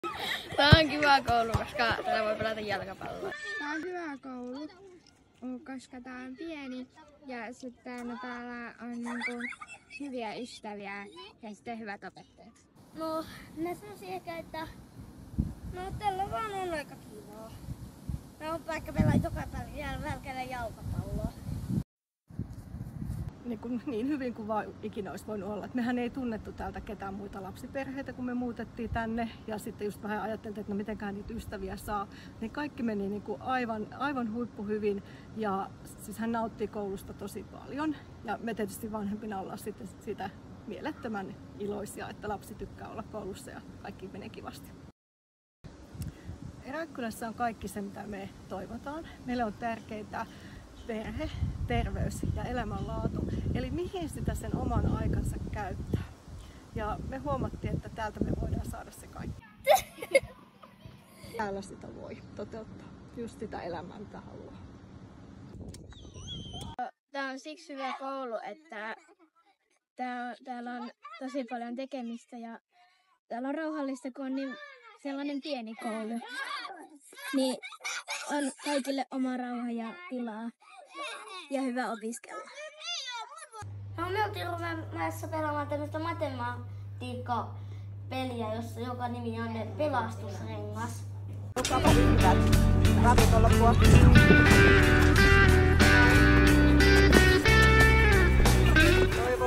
Jalkapalloa Rääkkylän koulussa välitunnilla
Rääkkylässä on pieni koulu, missä jokainen tuntee olonsa turvalliseksi ja arvostetuksi. Turvallisuus ei ole vain sana- se on arvo, joka ohjaa kaikkea koulun toimintaa.